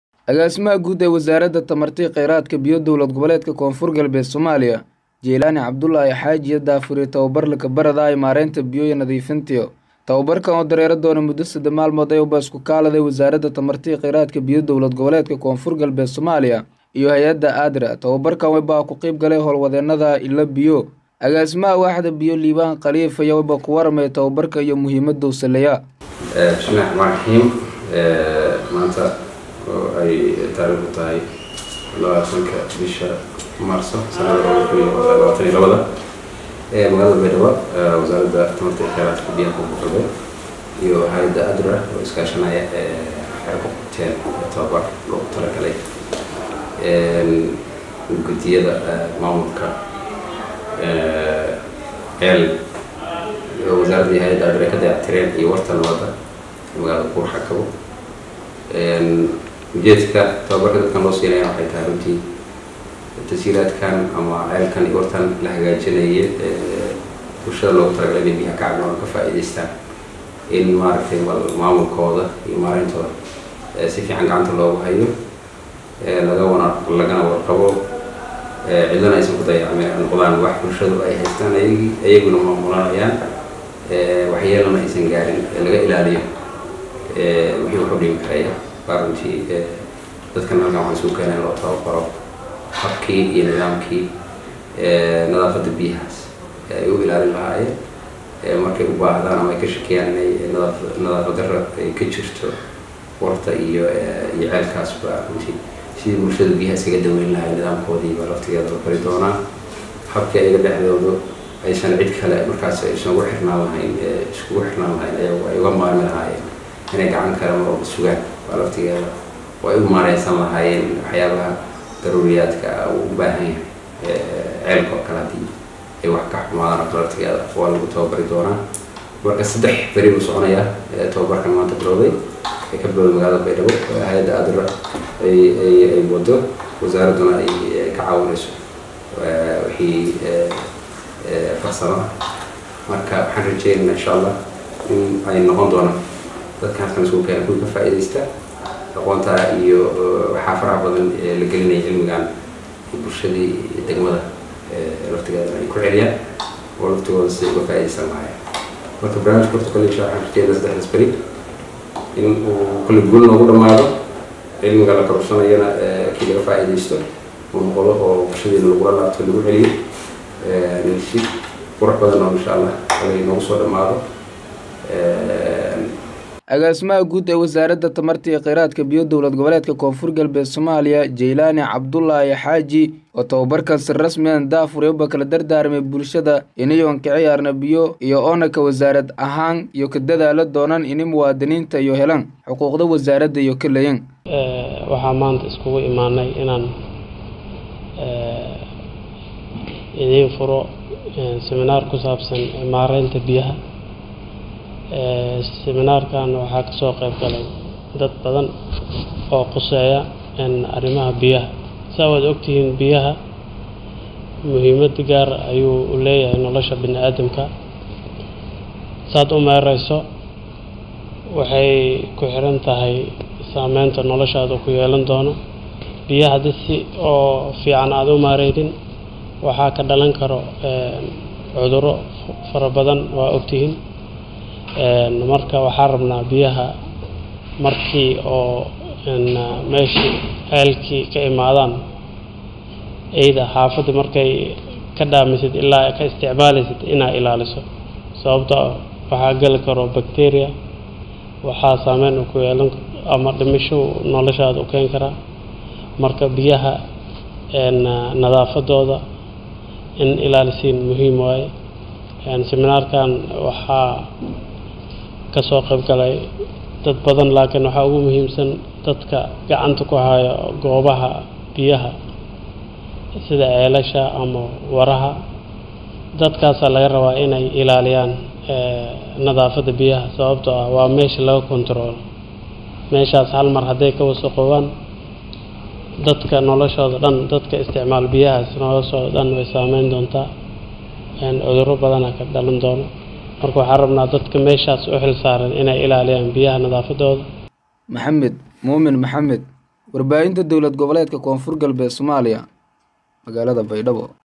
Agaasimaha Guud ee Wasaarada Tamarta iyo Kheyraad Biyaha DGKGS ayaa Daahfuray ........
Agaasimaha Guud ee Wasaarada Tamarta iyo Kheyraad Biyaha Dgkgs Jeylaani Abdullaahi Haaji ayaa Daahfuray Tababar Lagu Baranaayay Maaraynta Biyaha iyo Nadiifintooda